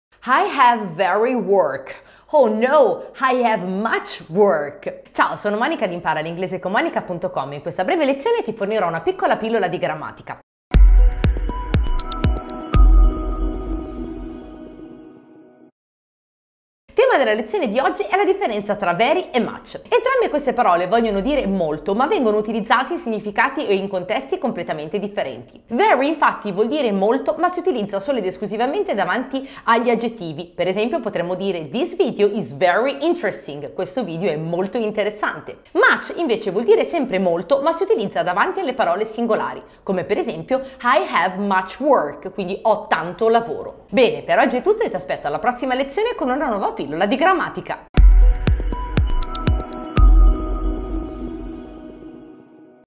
VERY vs MUCH: Pillola di Grammatica
[VERY vs MUCH]
Qual è la differenza tra very e much?
Guarda ORA il mio video e lo scoprirai! Ti aspetto martedì prossimo per una nuova pilloladiGRAMMATICA!

Riceverai per 4 giorni 1 lezione di estremo valore direttamente sulla tua email, a costo zero!